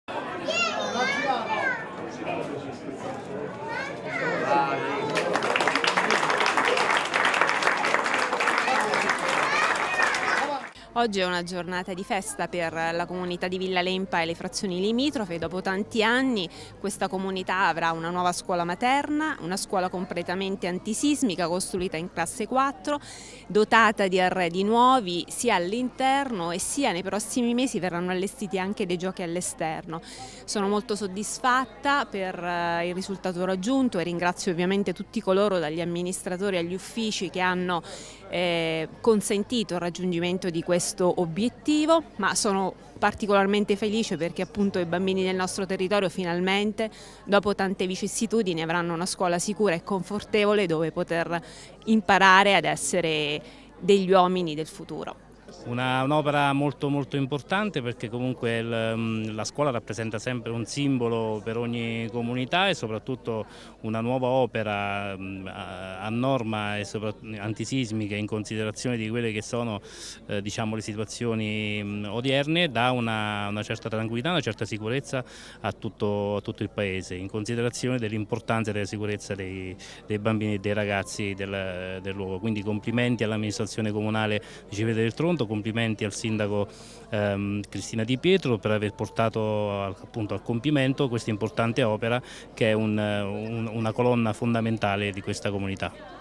Civitella del Tronto, inaugurata questa mattina la nuova scuola materna di Villa Lempa
| SUPER J |

Contatti Redazione:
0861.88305

Seguiteci su